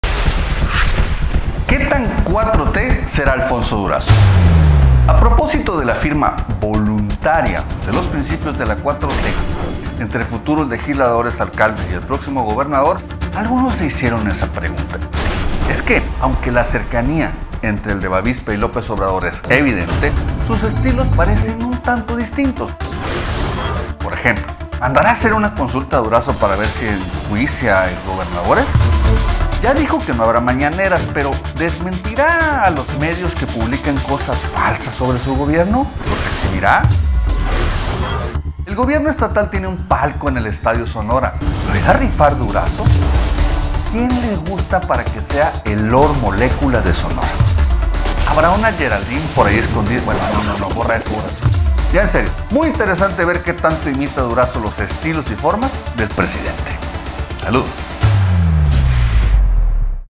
4T AL ESTILO SONORA
ESTILO AMLO/ESTILO DURAZO. El modelo de comunicación y estilo de gobierno de @lopezobrador_ ha sido exitoso, les guste o no a muchos. En 60Segundos analizamos la posibiidad de que @AlfonsoDurazo imite la fórmula presidencial en Sonora. Será? @Los60Segundos